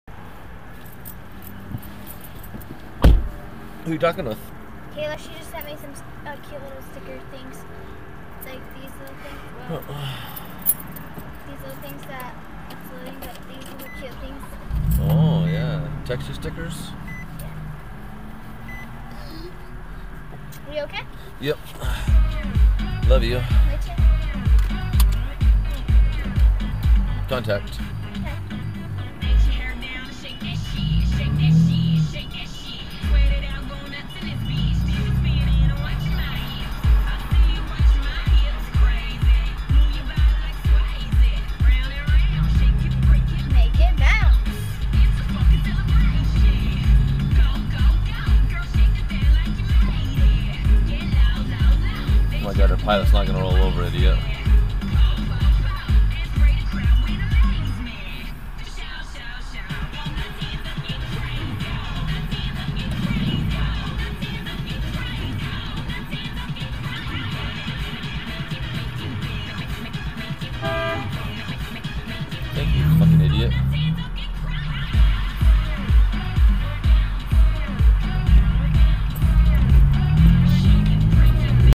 Guy on his phone driving thru the gas station
Seen this prime example of not paying attention to surroundings but rather your phone leaving my gas and go shop. Utah plates W97 0ES please pay attention, its not like my 6'8" truck is hard to hide, how you missed it is amazing upon itself.